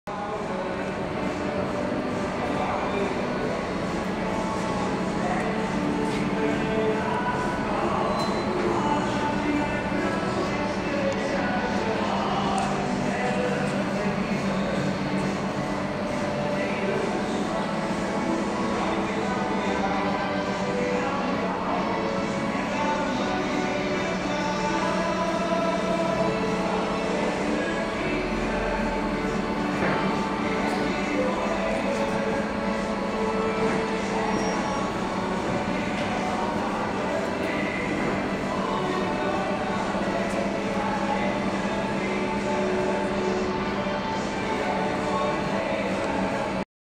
Opel Commodore GS Coupe 1969 (27)
Hij is weer in de originele kleur gespoten. Wanneer de achterkant matzwart  is en het hemeltje wit is gespoten kunnen we de commodore afbouwen